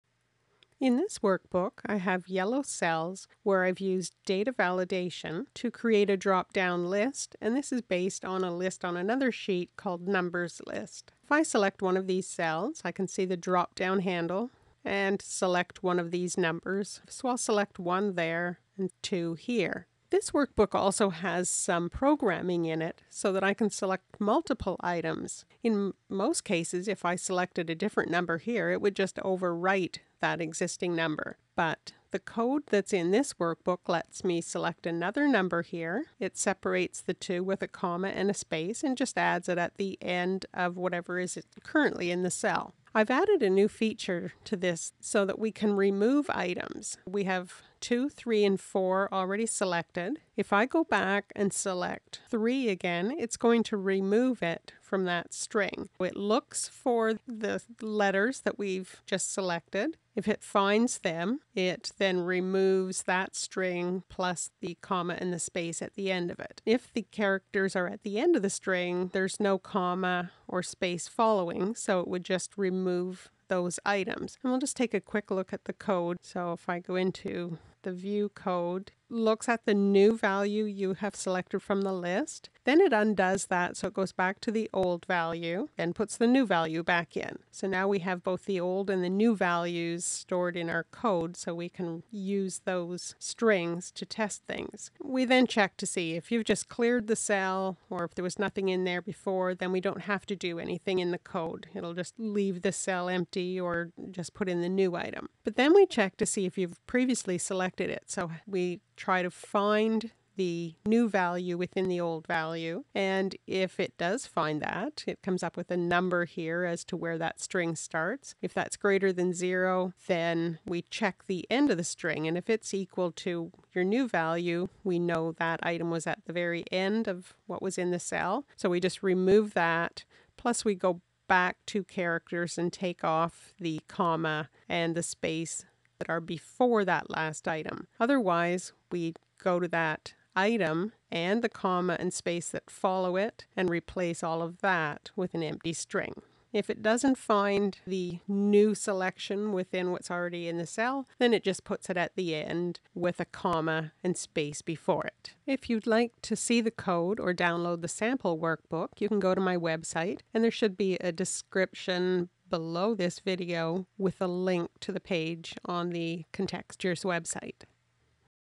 Excel Drop Down Multiple Select or Remove
With Excel data validation you can create drop down lists on a worksheet. In this example, you can select multiple items from the drop down list, and the new items are automatically added to the existing selections in the cell.

If an item has been selected before, in the current cell, that item will be removed, if you select it again.

This video shows how the multiple select and remove features works. You can also see the Excel VBA code, and the steps that it goes through, when you make a selection in the drop down list.

The code for this example is on the sheet named "SameCellAddRemove"